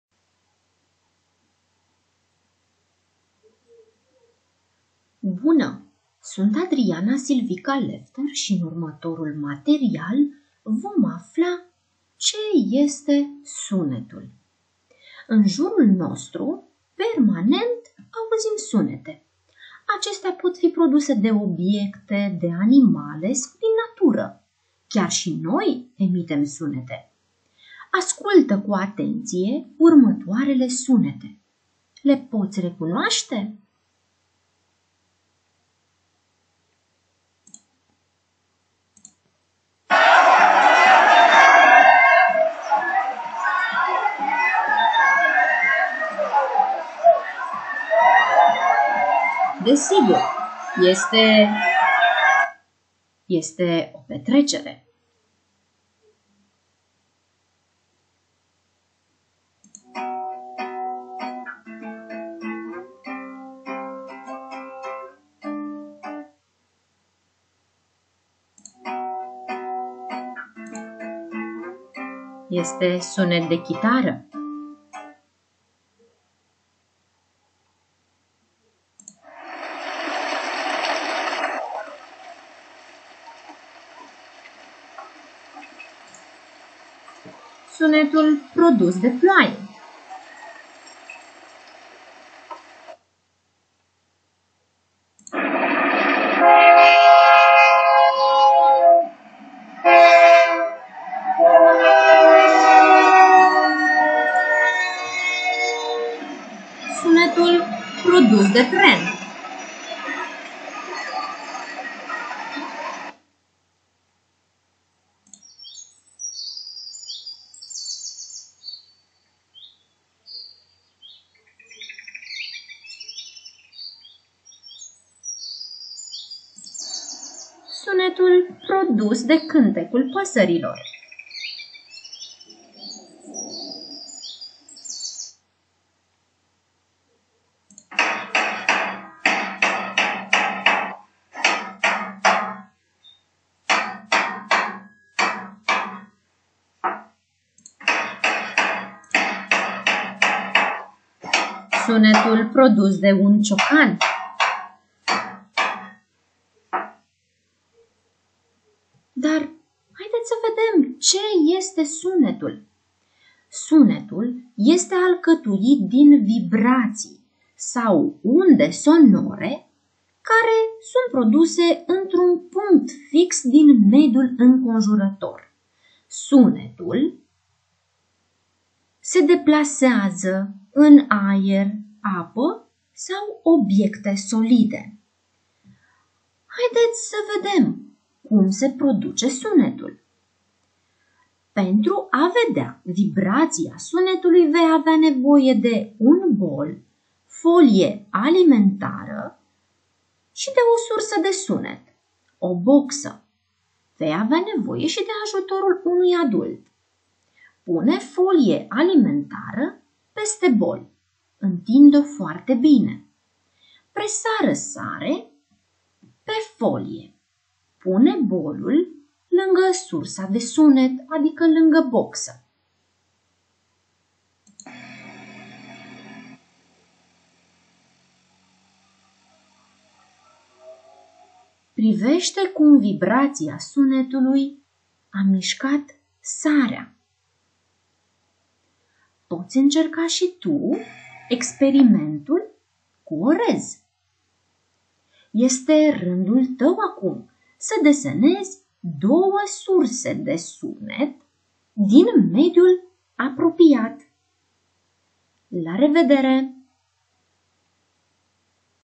Ce este sunetul?
Resursa educațională propusă este format din trei părți. La început, elevii vor identifica diverse sunete familiare din mediul înconjurător, apoi vor participa la un experiment care arată cum sunetul sau vibrația sa se transmite prin aer. La final vor desena două surse care produc sunete, la alegere.

Autor: Adriana Silvica Lefter